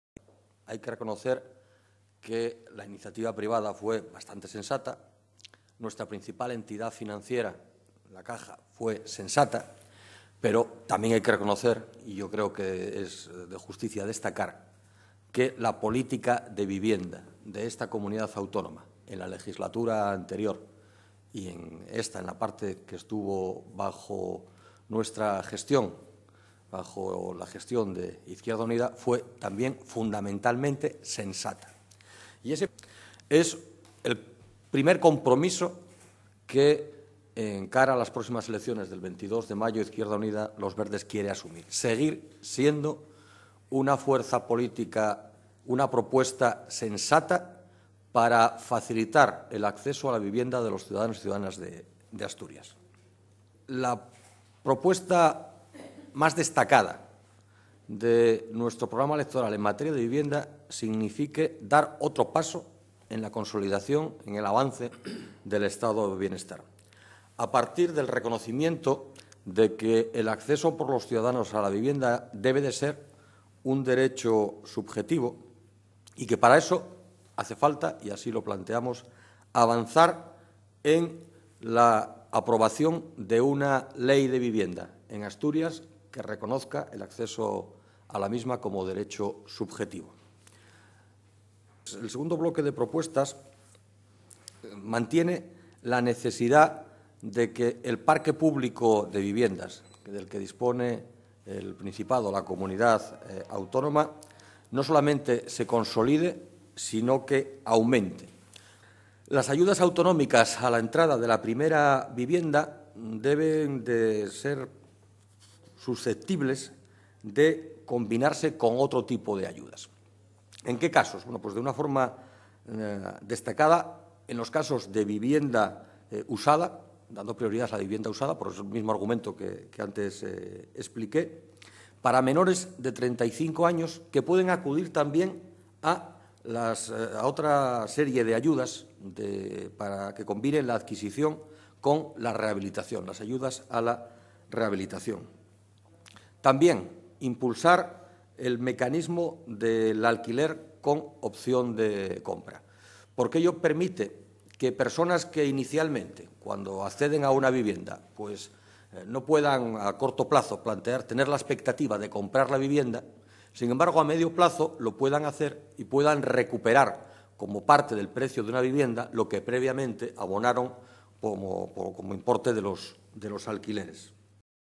Asturias de IU-Verdes presenta candidatura
Declaraciones de Jesús Iglesias, candidato a la presidencia de Asturias de IU-Verdes quien presenta el programa electoral en materia de vivienda acompañado de Manuel G. Orviz, Director General de Vivienda, Francisco Santianes, candidato número dos de IU-Verdes a la alcaldía de Gijón y Cristina Tuya